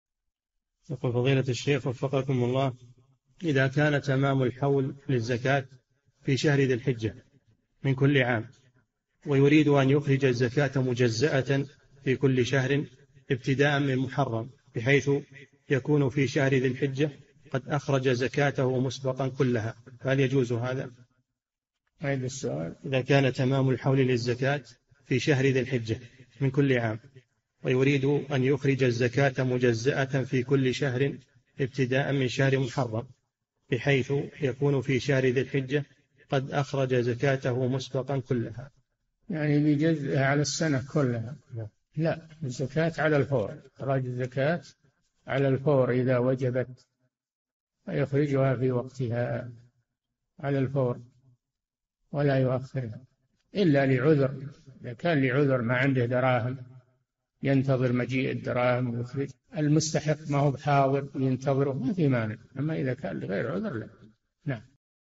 [3627- 4717] هل يجوز إخراج الزكاة مجزأة على السنة كلها؟ - الشيخ صالح الفوزان
"[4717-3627] إذا كان تمام الحول للزكاة في شهر ذي الحجة من كل عام ويريد أن يخرج الزكاة مجزئة في كل شهر ابتداء من محرم بحيث يكون في شهر ذي الحجة قد أخرج زكاته مسبقًا كلها فهل يجوز هذا؟
الجَوَابُ: يعني يجزئها على السَّنة كلها لا، الزكاة على الفور إخراج الزكاة على الفور إذا وجبت فيخرجها في وقتها على الفور ولا يؤخرها إلا لعذر إذا كان لعذر ما عنده دراهم ينتظر مجيء الدراهم ويخرجها، المستحق ما هو حاضر وينتظره ما في مانع، أما إذا كان لغير عذر لا.

للاشتراك في قناة الشيخ صالح الفوزان
فتاوى المجالس في تفسير المفصل

للاستفسار والإقتراحات والدعم
رئيس الفريق
لمشروع كبار العلماء